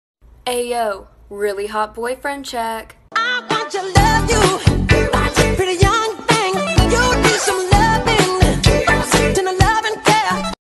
Lina showing off her boyfriend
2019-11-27

Chase Matthews was Lina's squib boyfriend before he was killed in a deatheater attack.